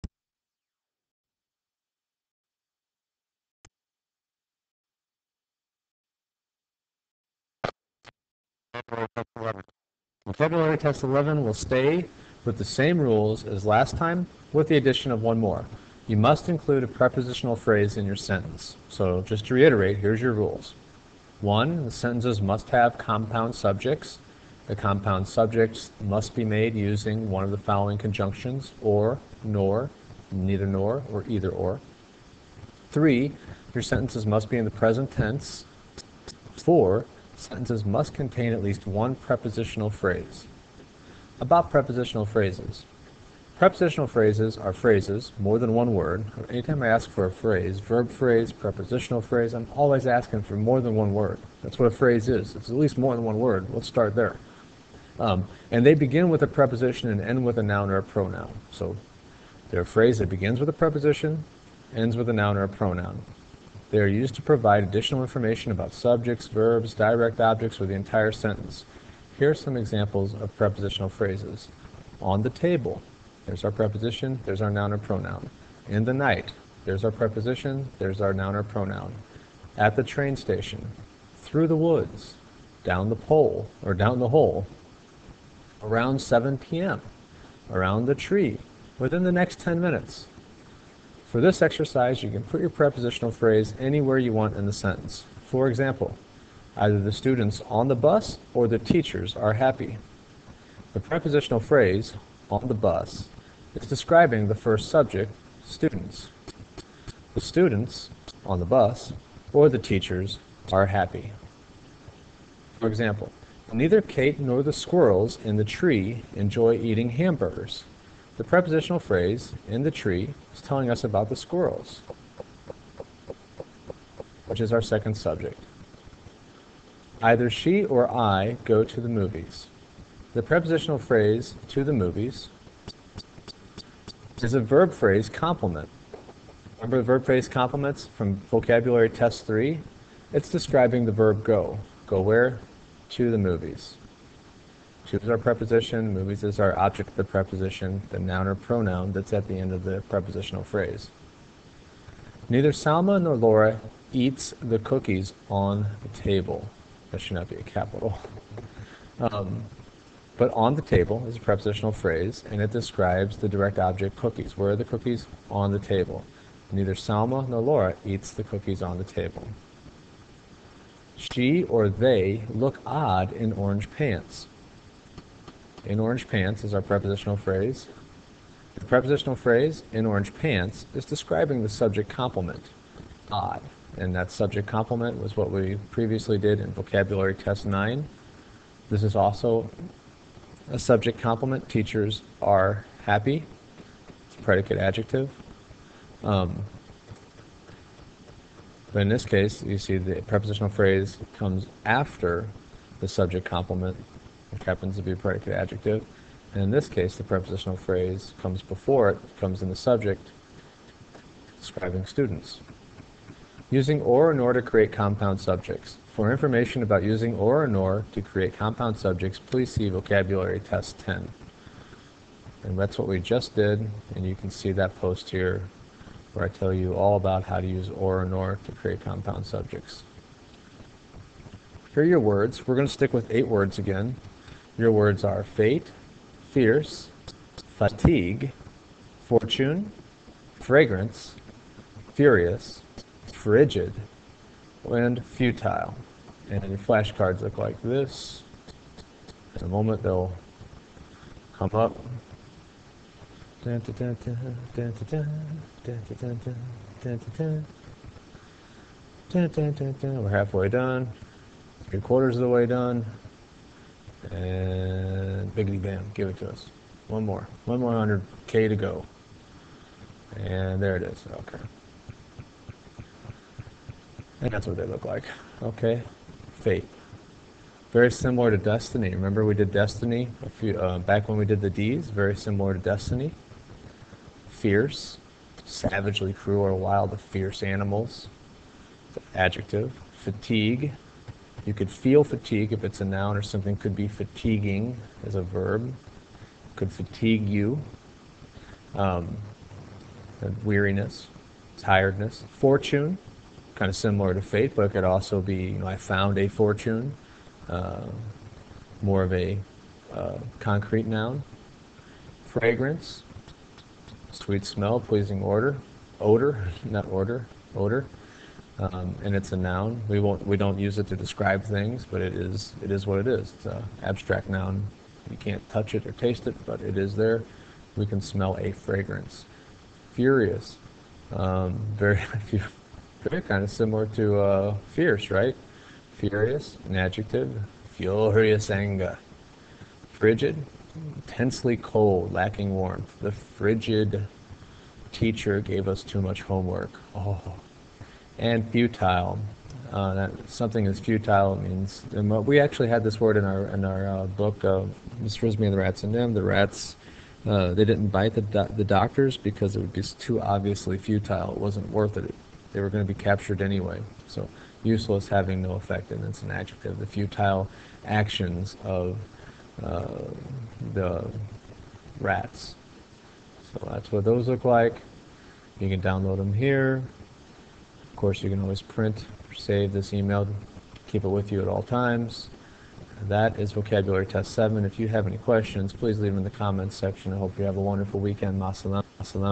Vocabulary Test 11
Mr. Clements discusses the rules for Vocabulary Test 11.